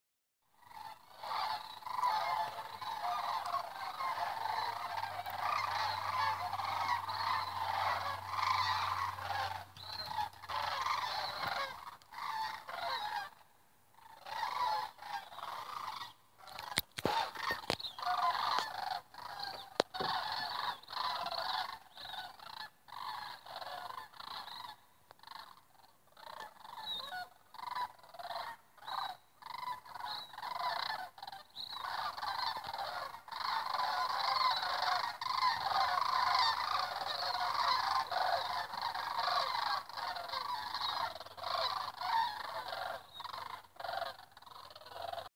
A lot of Herons
Watching Herons flying in the sky